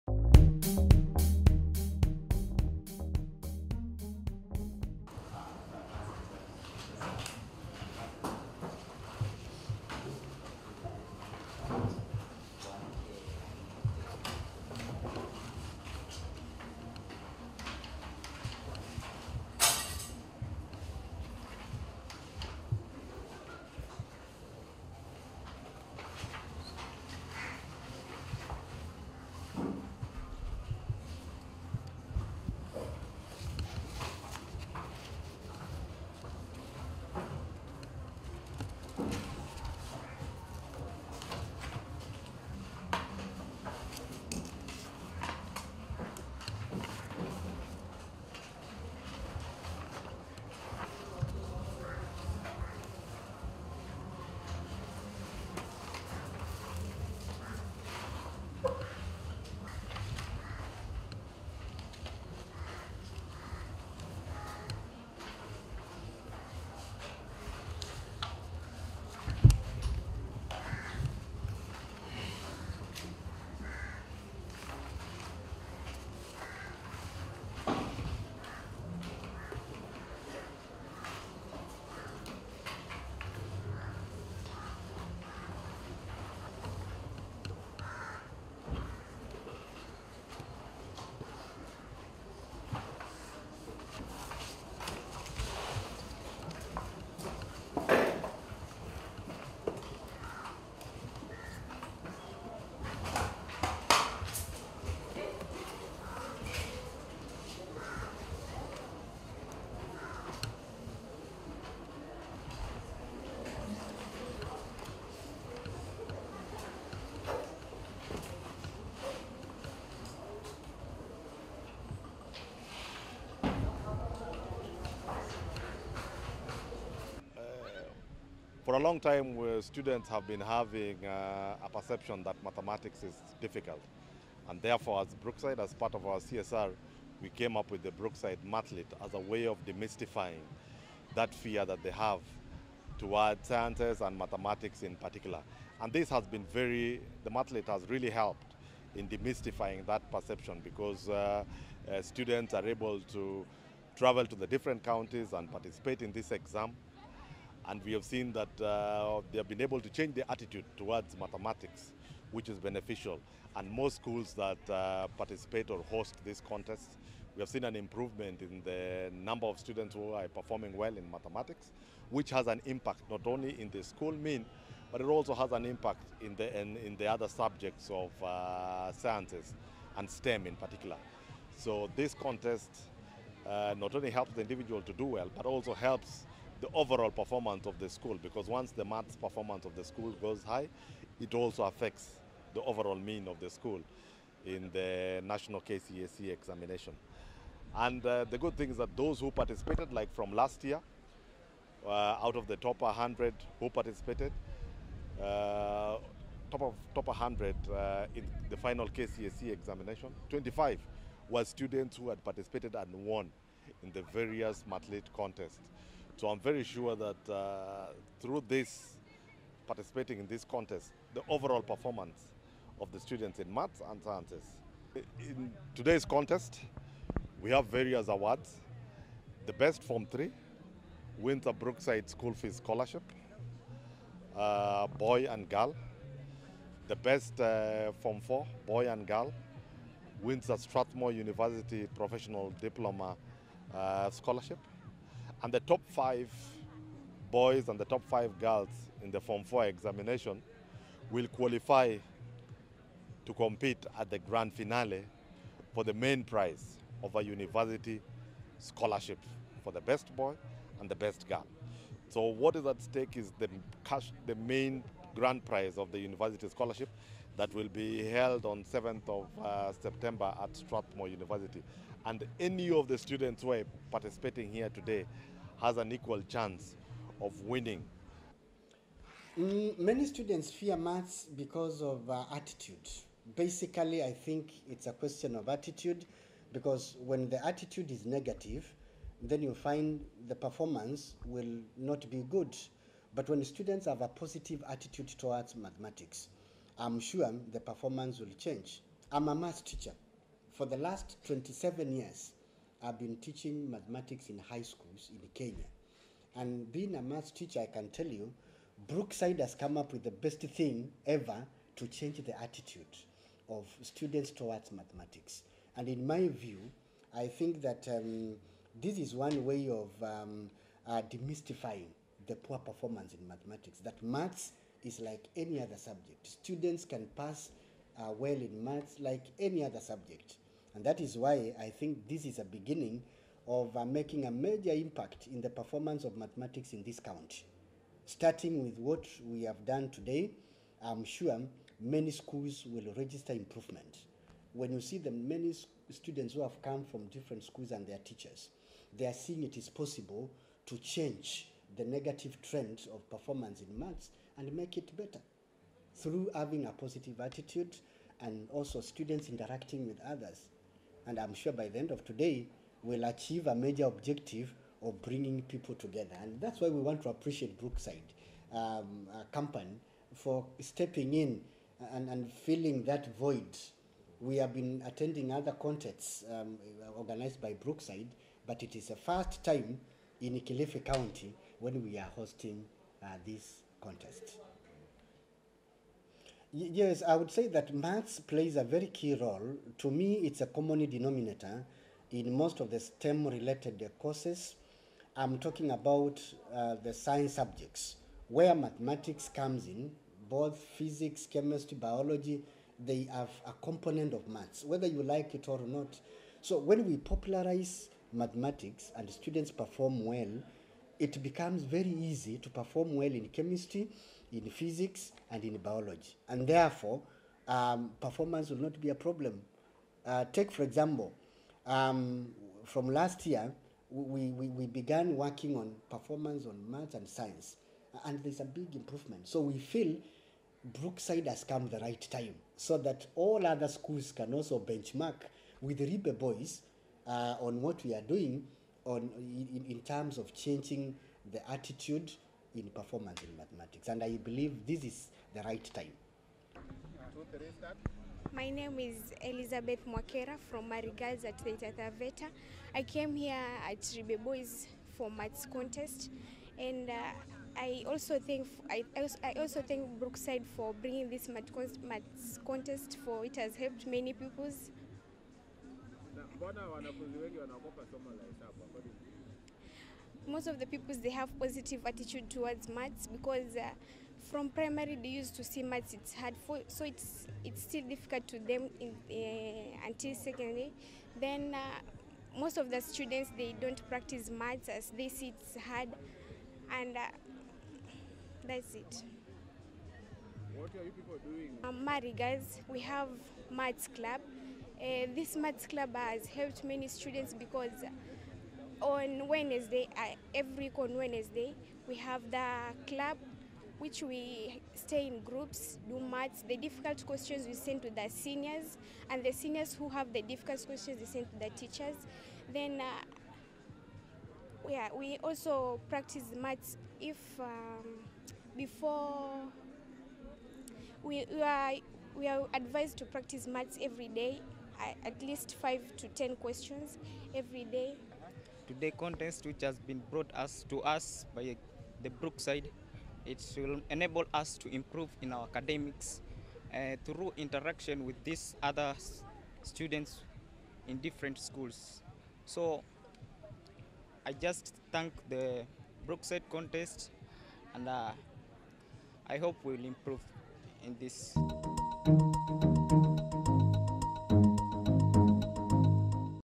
mathematics contest
Milk processor Brookside Dairy has held mathlete contest in Ribe Boys Secondary school in Kilifi county aimed at improving learning outcomes in mathematics in secondary schools. # mathematics#contest